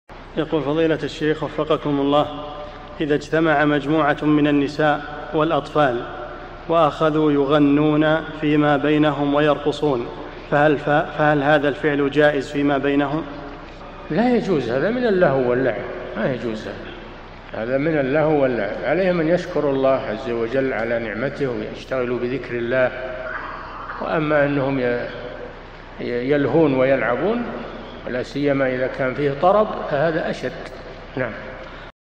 [682 -1024] حكم اجتماع النساء والأطفال مع الغناء والرقص - الشيخ صالح الفوزان
-[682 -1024] السؤال: إذا اجتمع مجموعة من النساء والأطفال، وأخذوا يغنون فيما بينهم، ويرقصون، فهل هذا الفعل جائز فيما بينهم؟
الجواب: لا يجوز هذا من اللهو واللعب عليهم أن يشكروا الله عز وجل على نعمته ويشتغلوا بذكر الله وأما أنهم يلهون ويلعبون ولاسيما إذا كان في طرب فهذا أشد!
للاشتراك في قناة الشيخ صالح الفوزان
سؤال وجواب من شرح الفتوى الحموية ( 1024 ) مقطع

للاستفسار والإقتراحات والدعم
رئيس الفريق
لمشروع كبار العلماء